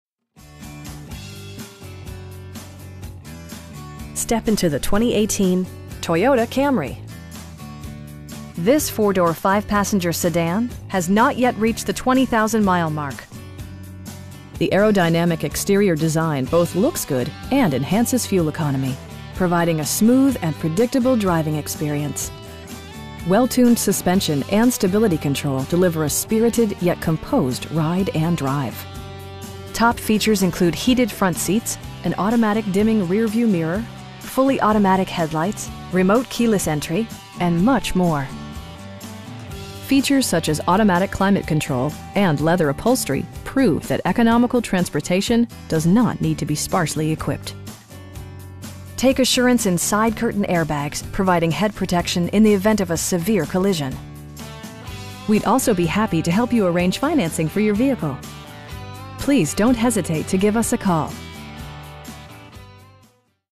2018 Toyota Camry XSE in Salem, OR 97301
Capitol Toyota
783 Auto Group Avenue Ne

Step into the 2018 Toyota Camry. This 4 door, 5 passenger sedan has not yet reached the 20,000 mile mark! The aerodynamic exterior design both looks good and enhances fuel economy, providing a smooth and predictable driving experience. Well tuned suspension and stability control deliver a spirited, yet composed, ride and drive Top features include heated front seats, an automatic dimming rear-view mirror, fully automatic headlights, telescoping steering wheel, remote keyless entry, cruise control, an overhead console, and much more. Features such as automatic climate control and leather upholstery prove that economical transportation does not need to be sparsely equipped. Take assurance in side curtain airbags, providing head protection in the event of a severe collision. We'd also be happy to help you arrange financing for your vehicle. Please don't hesitate to give us a call.